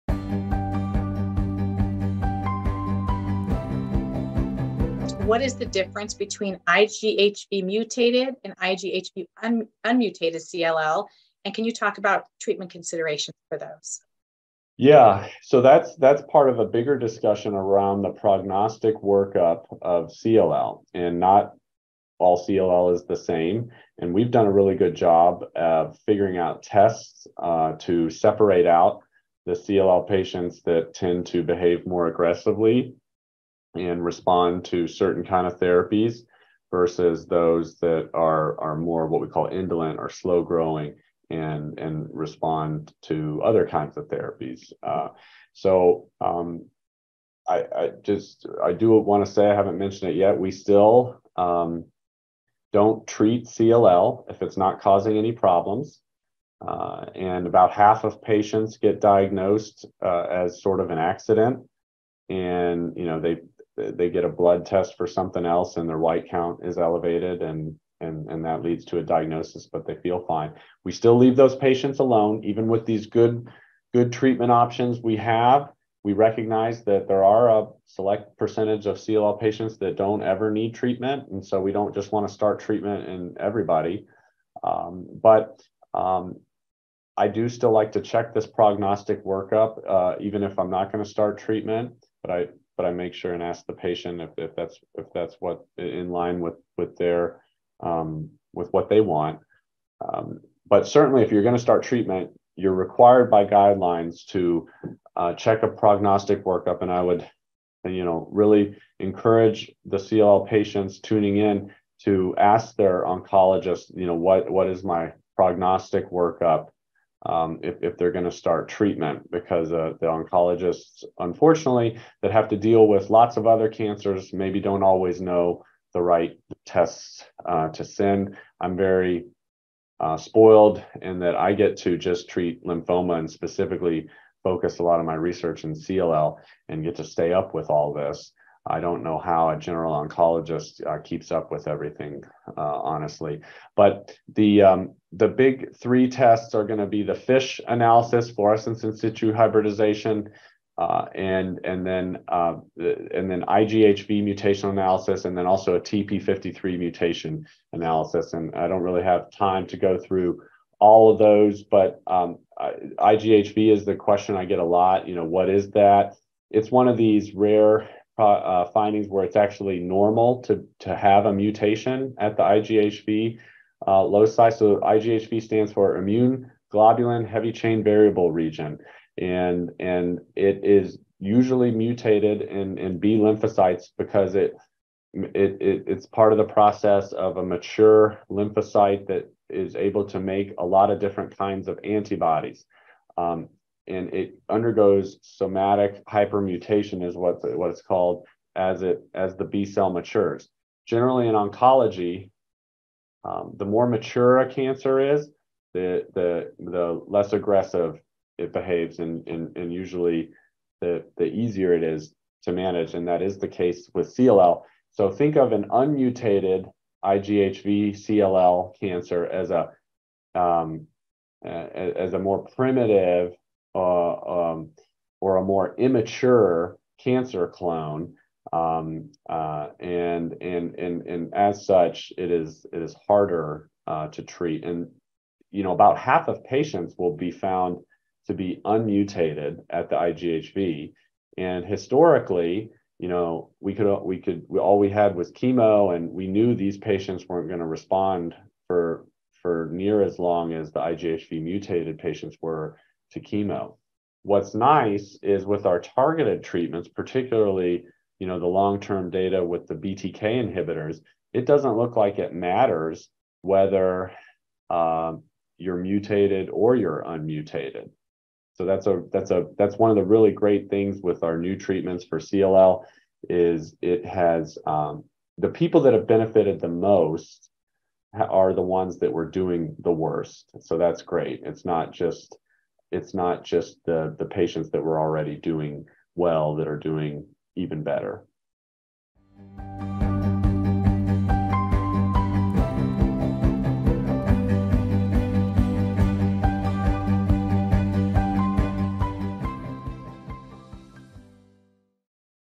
IGHV-Mutated vs IGHV-Unmutated CLL | What’s the Difference?
What’s the difference between chronic lymphocytic leukemia (CLL) that’s IGHV-mutated versus IGHV-unmutated? Expert Dr. Ryan Jacobs from Levine Cancer Institute explains tests involved in CLL diagnosis and treatment options.